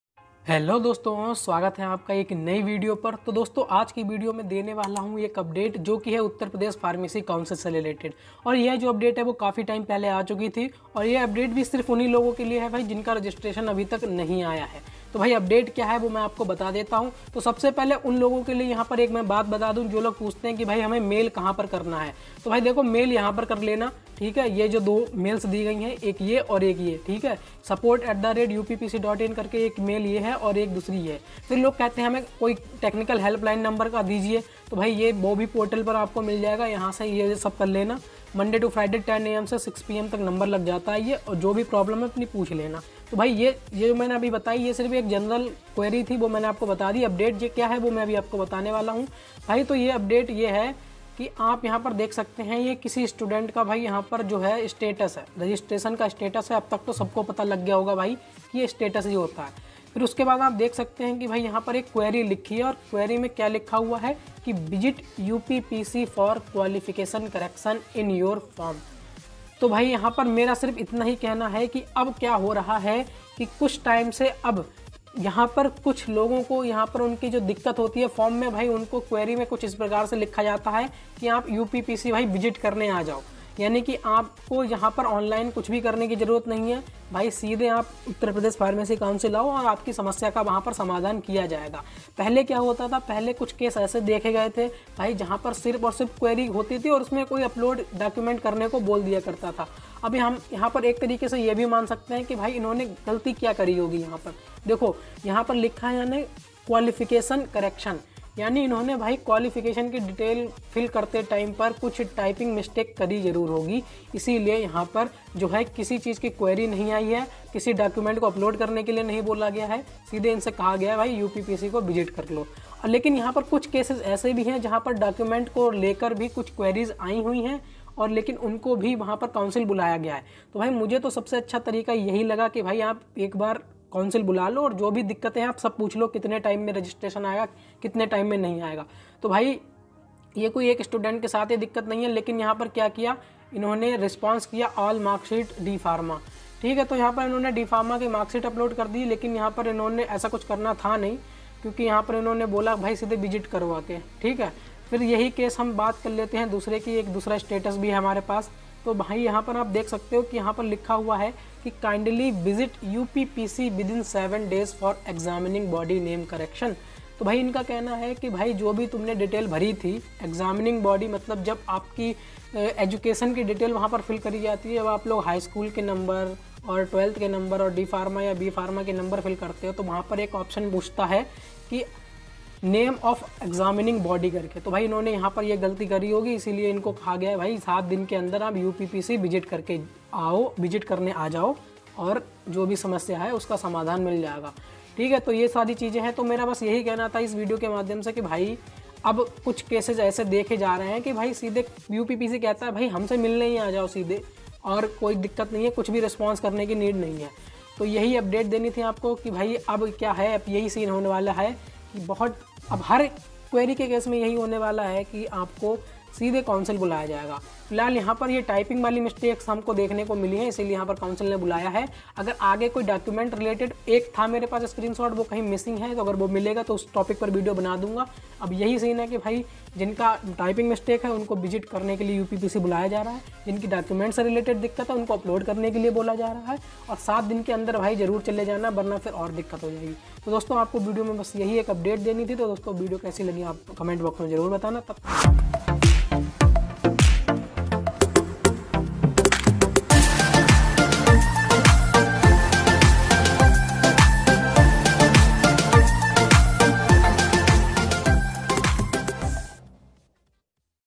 UP Pharmacy Council New Update 2023 || UP Pharmacy council Online Registration
UP Pharmacy Council New Update 2023 ||
UP Pharmacy council Online Registration ||

👉This is Akshay dosto is video Me mne apko UP Pharmacy Council ke New Update Ke bare me Bataya hain.